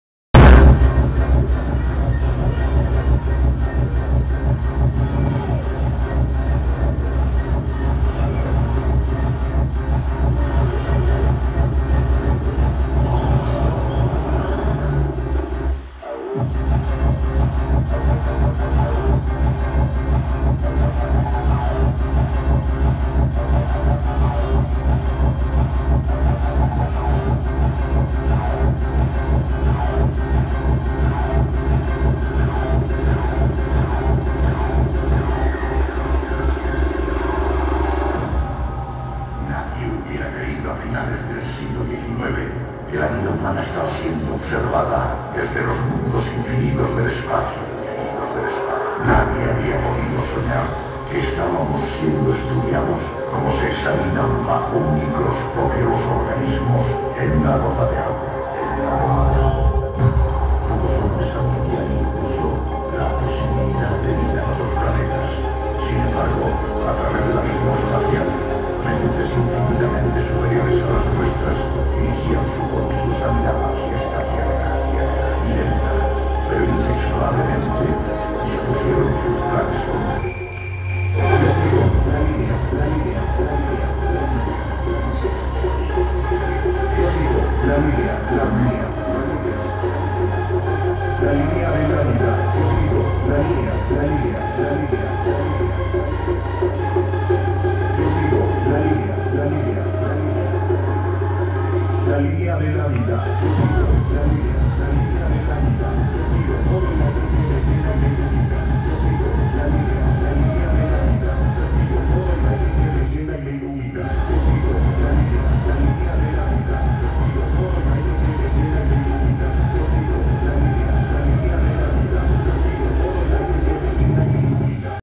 DJ Riki XL Time to Machine + Aniversario Destruxion Nit i Dia (Vic) 4/4/2014
una parte de la sesión de DJ Riki XL del festival Time to Machine + Aniversario Destruxion en la sala Ni i Dia de Vic